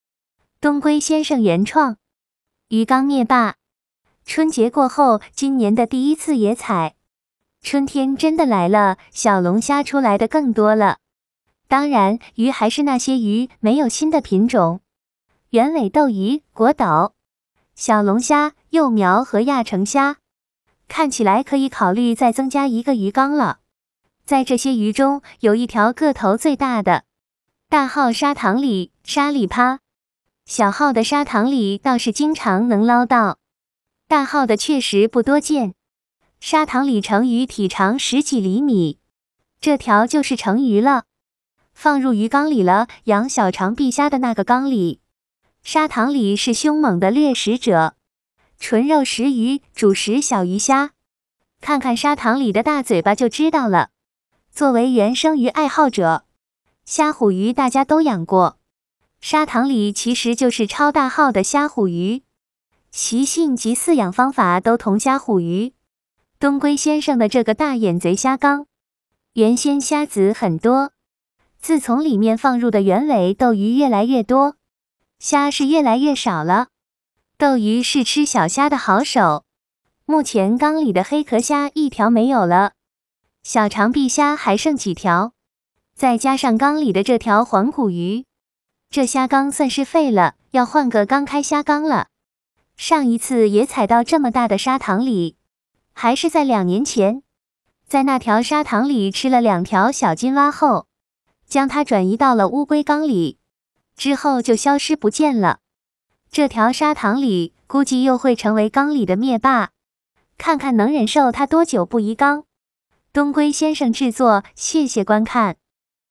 魚缸裏的滅霸！ 野采原生小型魚視頻，收穫大號沙塘鱧沙裡趴魚一條
科普型、實踐派，烏鶇、烏龜和原生魚，古法低成本生態飼養經驗分享。 【注意】：鶇龜先生無網店、無合夥人、不賣貨，謹防受騙！